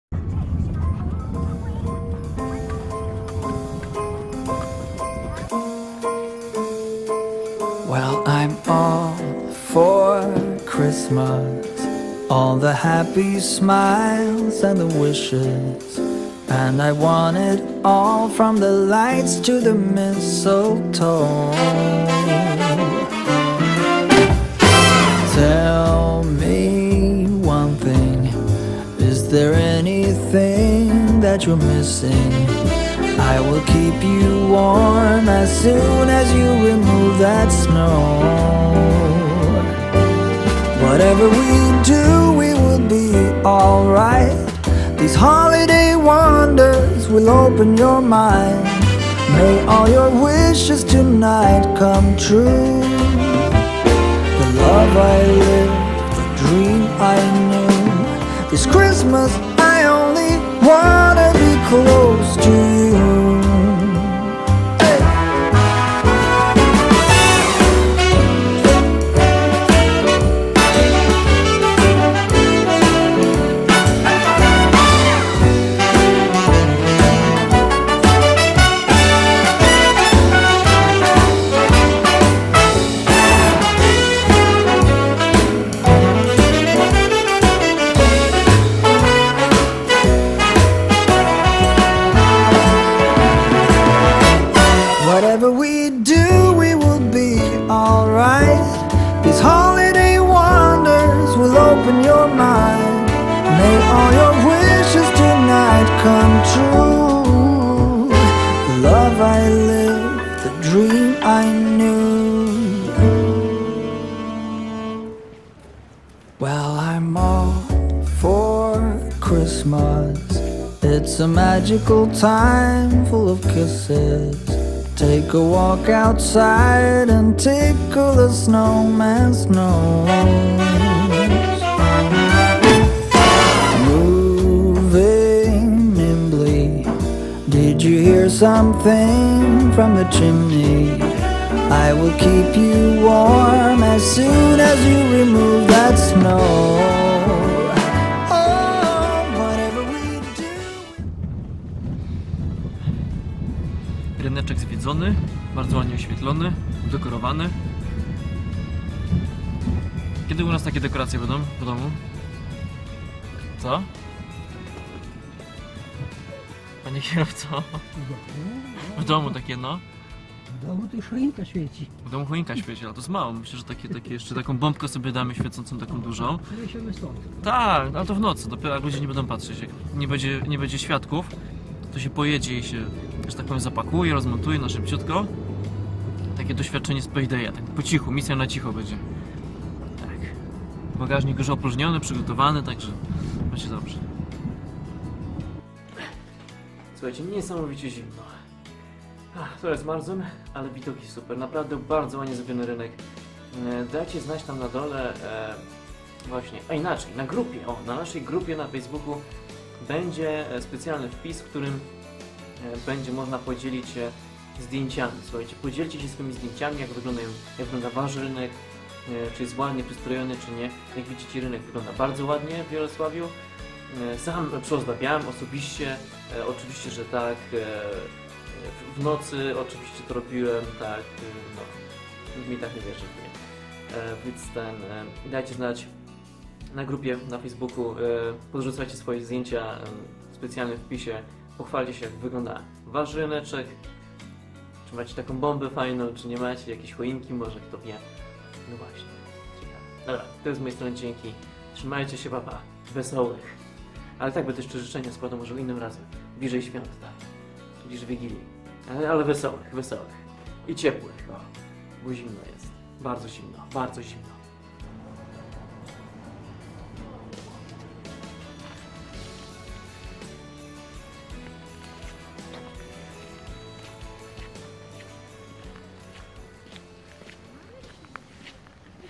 WESOŁYCH Wesołych, Jarosław 360 /VLOG
Świąteczny klimat wpadł na rynek w Jarosławiu. Postanowiłem podejrzeć jak to wygląda i podzielić się z Wami w postaci nagrania 360 stopni 4K

►Jak Wygląda Wasz Rynek? Pokaż tu